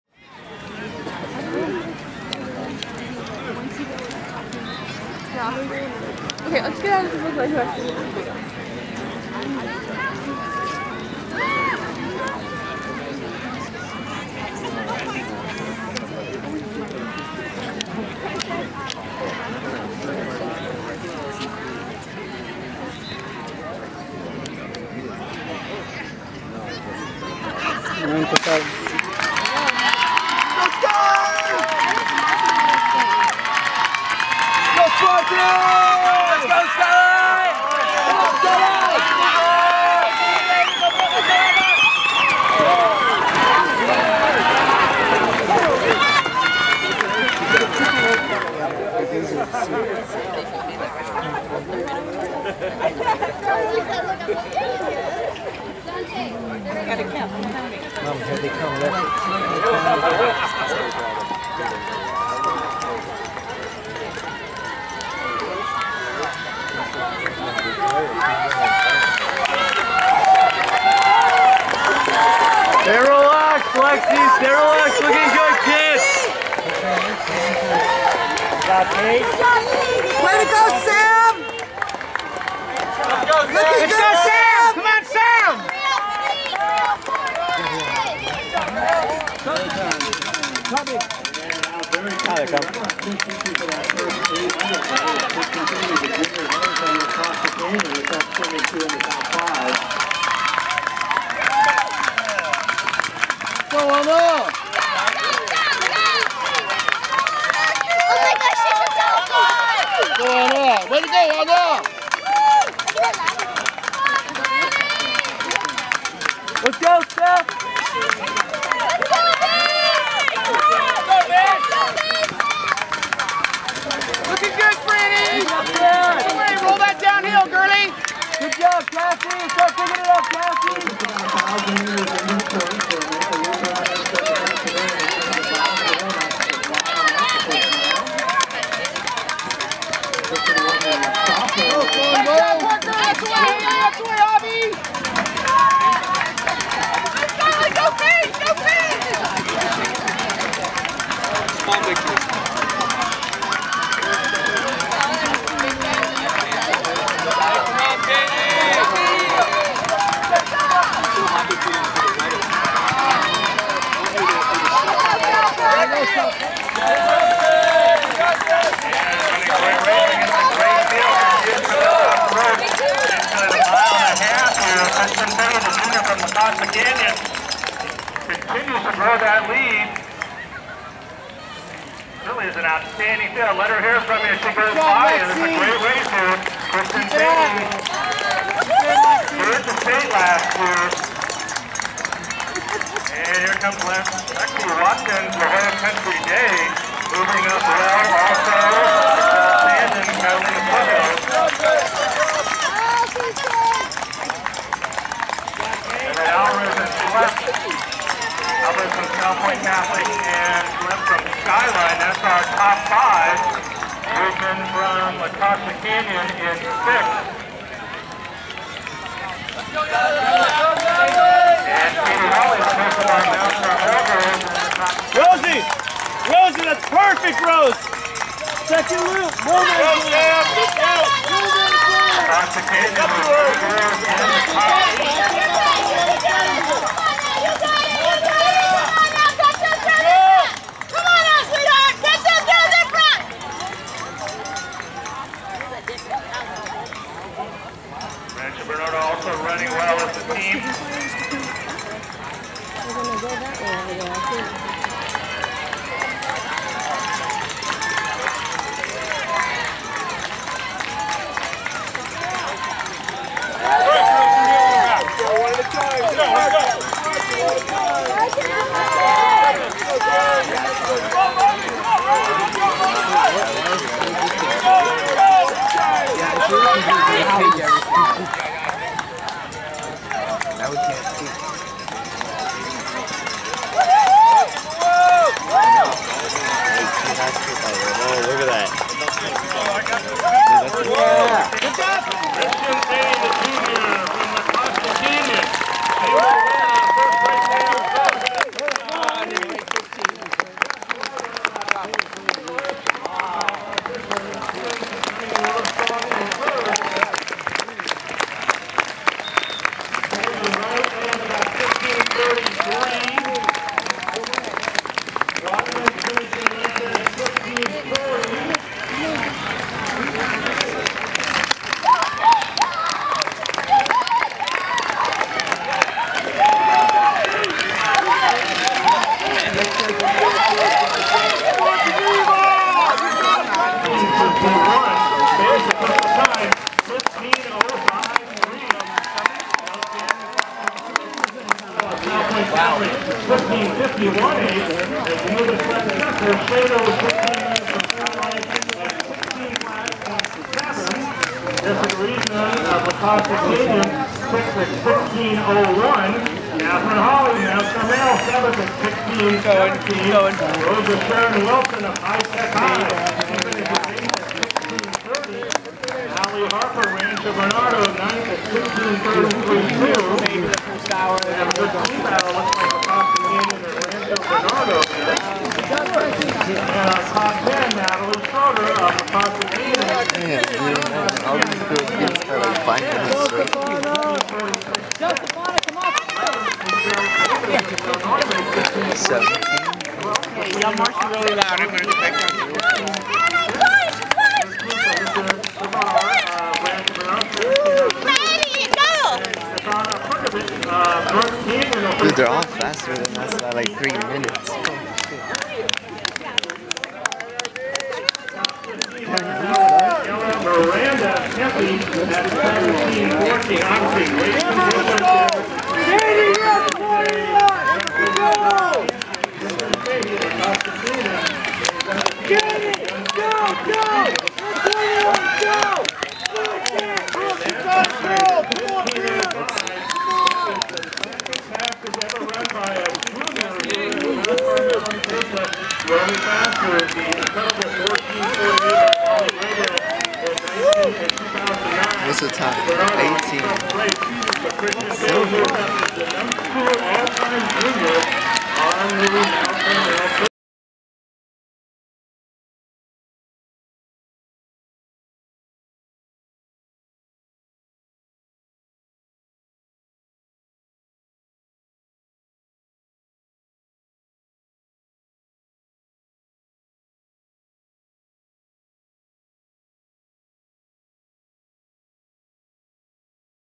17' Mt Carmel XC Invite Girls D2 Senior/Invitational
38th Annual Mt Carmel XC Invitational-Girls D2 Senior/Invitational
2.75 miles
Morley Field, 9/16/17

Top 10 Girls
1. 11 Kristin Fahy 15:05.3 La Costa Canyon
2. 12 Richelle Sandin 15:34.6 Catalina Foothills
3. 12 Lexi Watkins 15:42.8 La Jolla Country Day
4. 12 Ana Alvarez-Tostado 15:51.8 Salpointe Catholic
5. 12 Geneva Schlepp 16:00.7 Skyline
6. 11 Jessica Riedman 16:01.3 La Costa Canyon
7. 12 Katherine Hawley 16:17.2 Mt Carmel
8. 12 Rose of Sharon Wil... 16:30.4 High Tech High
9. 12 Ali Harper 16:33.2 Rancho Bernardo
10. 11 Natalie Schroeder 16:46.6 La Costa Canyon